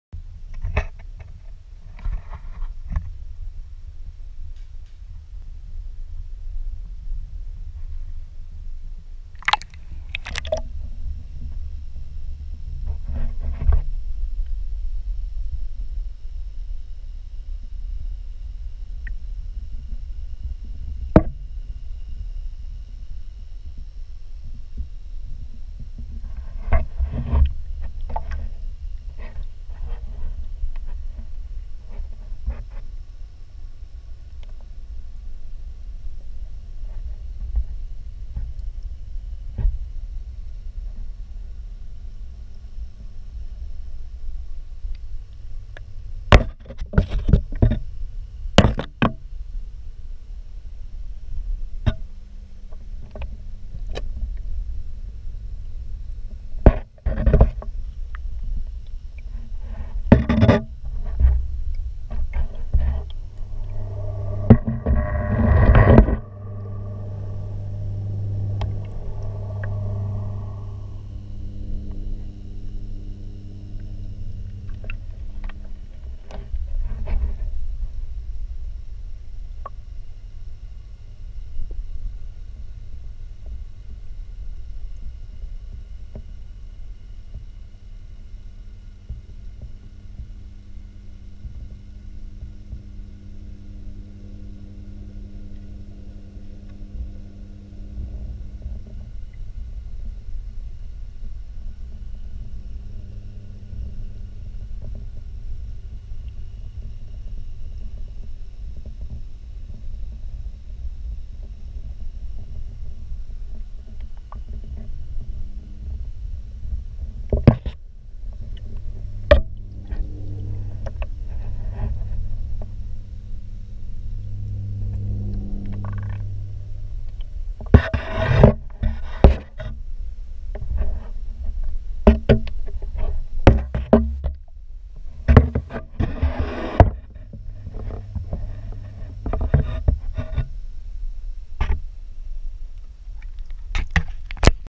Gopro immersion alligator gar 3000 liters 2 + Led
+ Led video en immersion, alligator gar, lepisosteus oculatus, et vieja breidohri reproduction